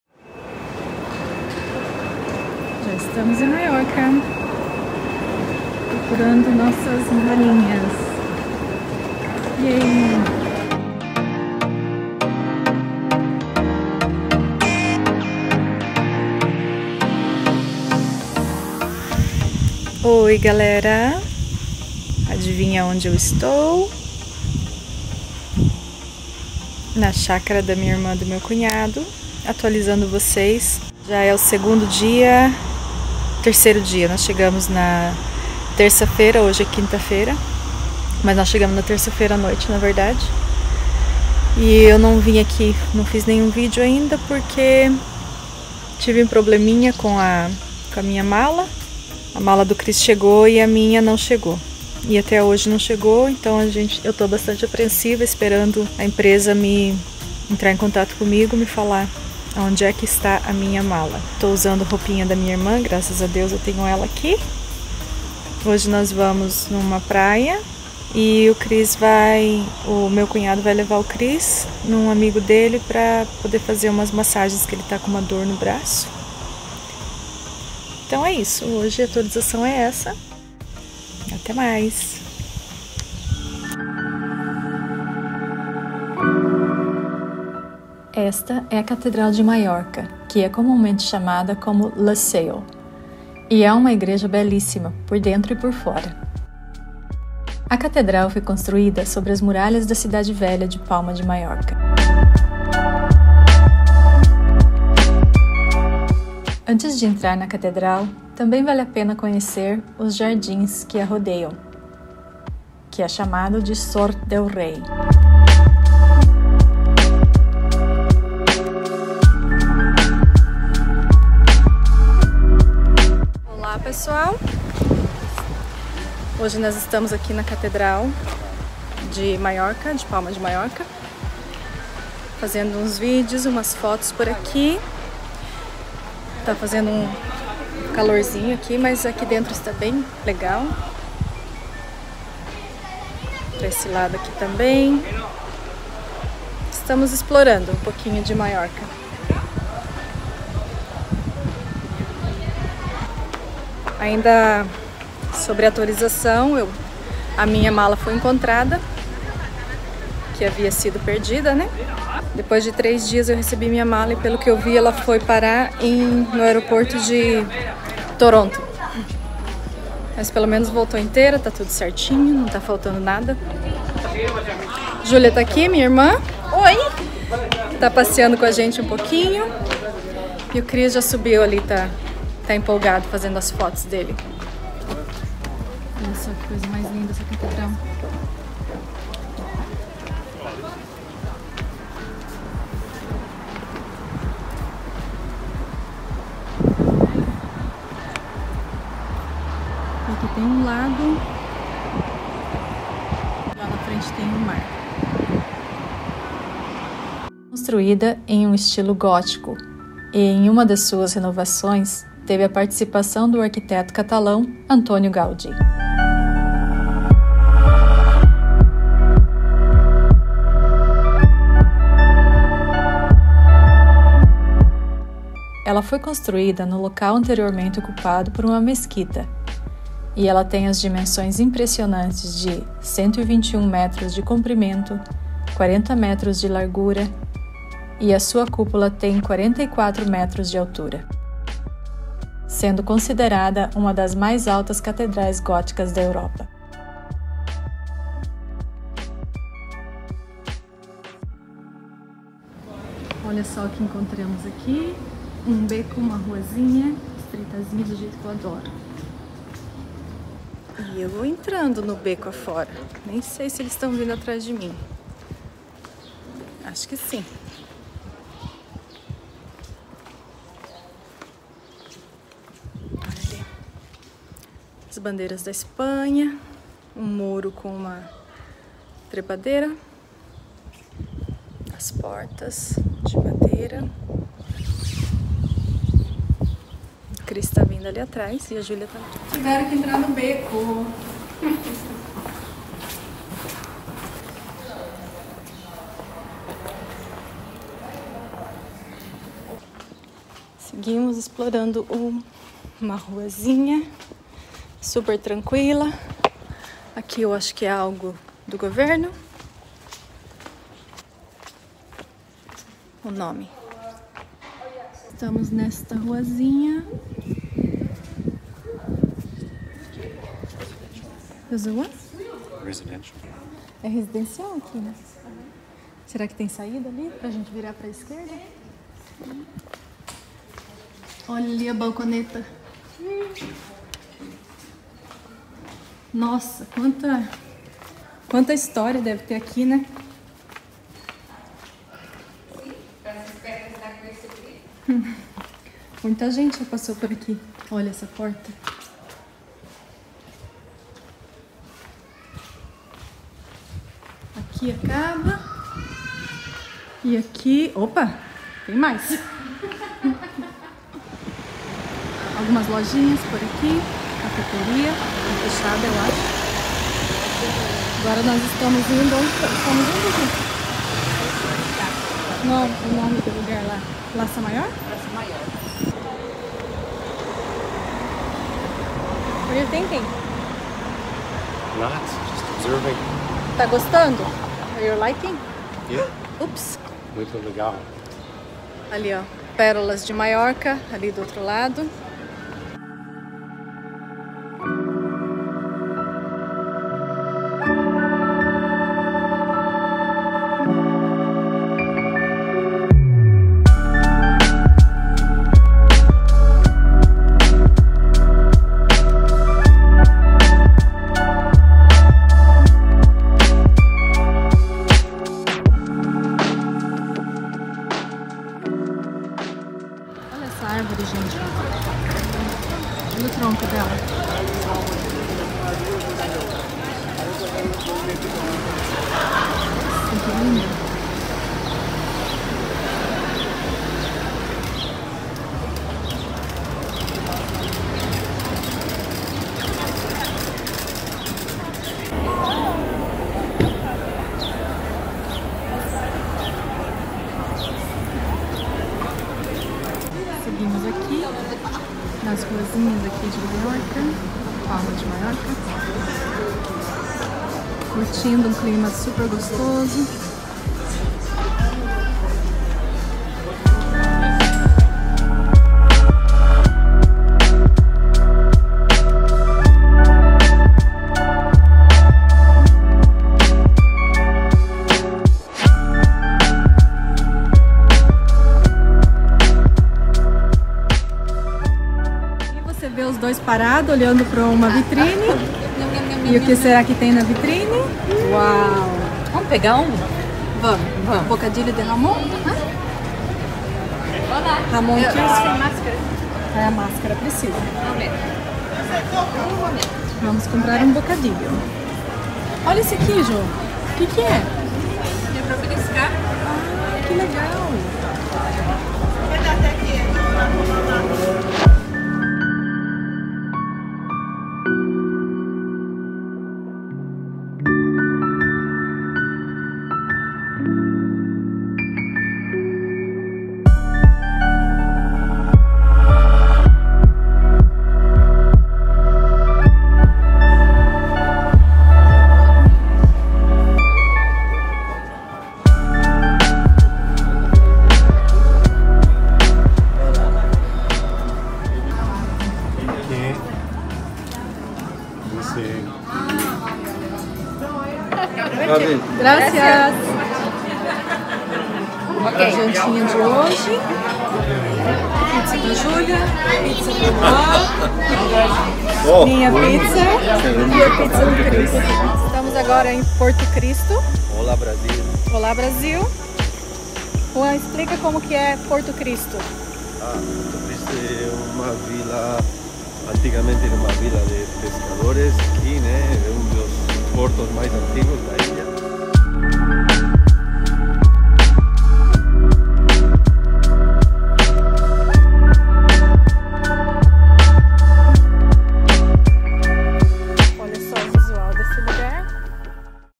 Ep. 02: TURISTANDO EM PALMA ❤️+ ÁRVORE DE MAIS DE 500 ANOS!
Apesar de ja ter morado em Mallorca eu não canso de ficar encantada com sua historia e beleza.